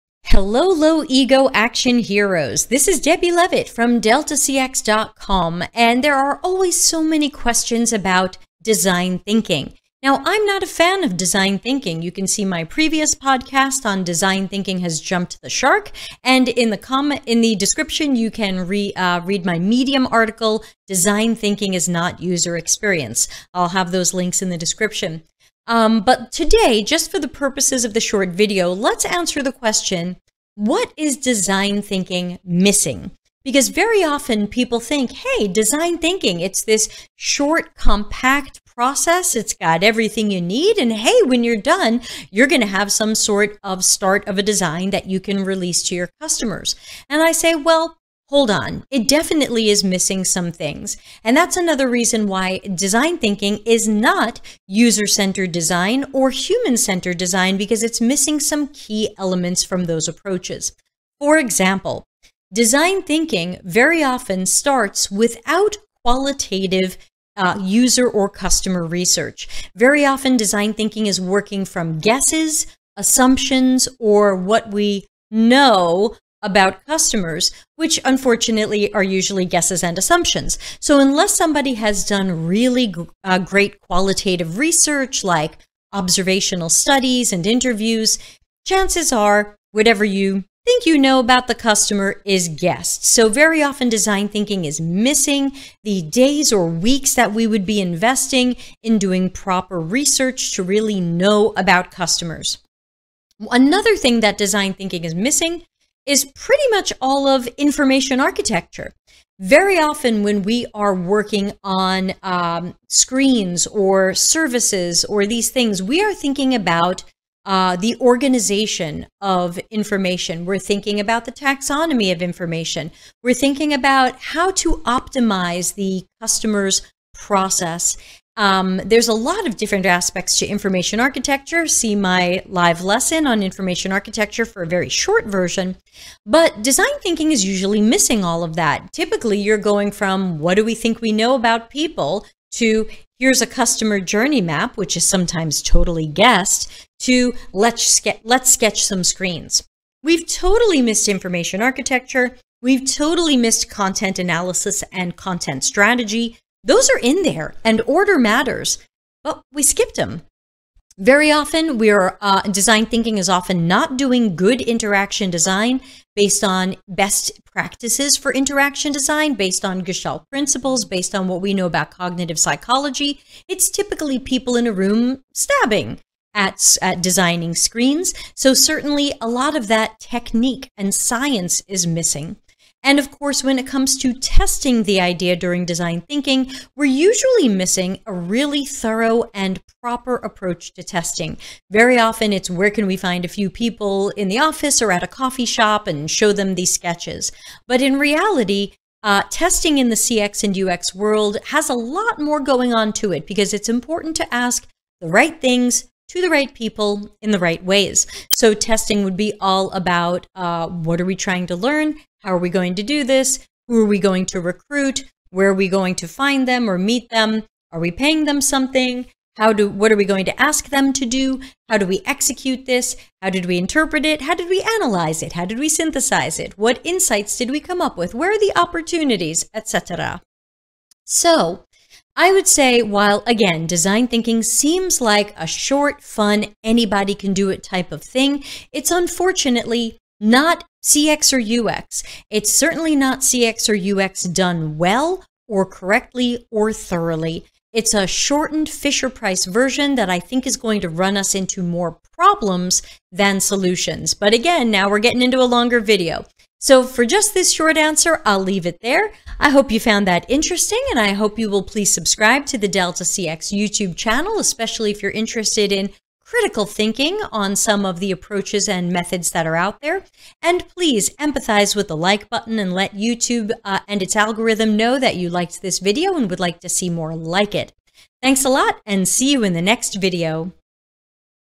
What is design thinking missing? Is design thinking the same as a UX process?
It's missing a lot.